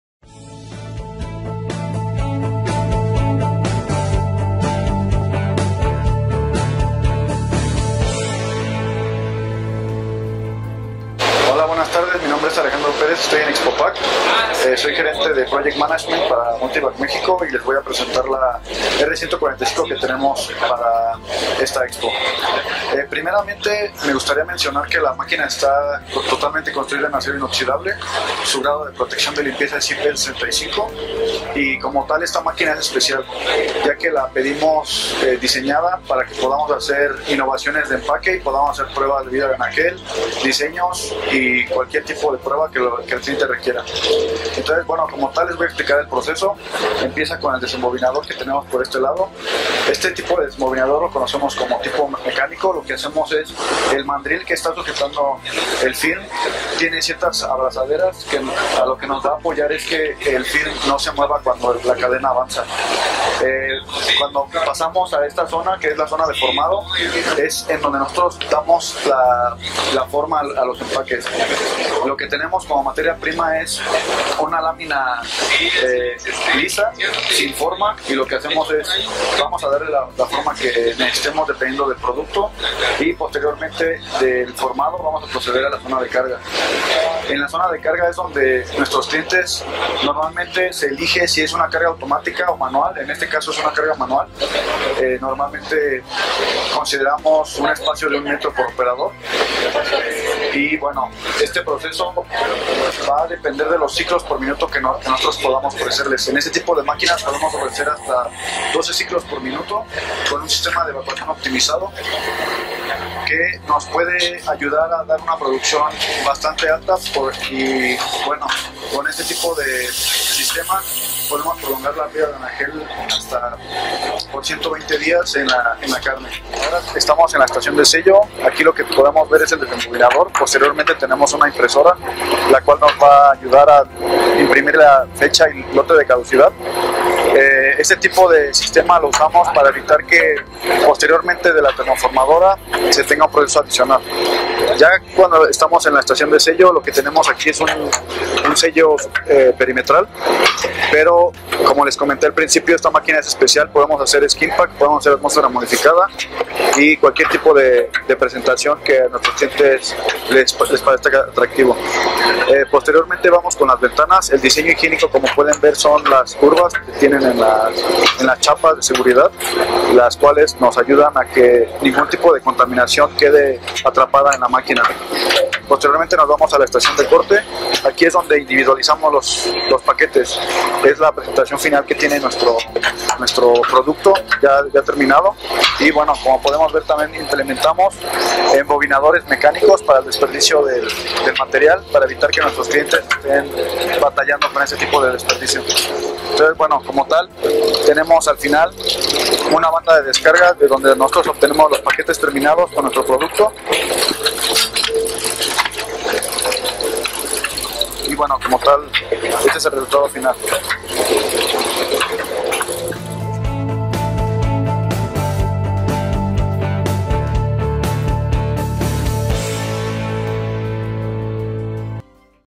Multivac en Expo Pack México 2018
En el marco de Expo Pack México 2018, Alejandro Pérez, Gerente de manejo de proyectos de Multivac México, presentó para El Empaque+Conversión el novedoso equipo R145 para la fabricación de empaques termoformados.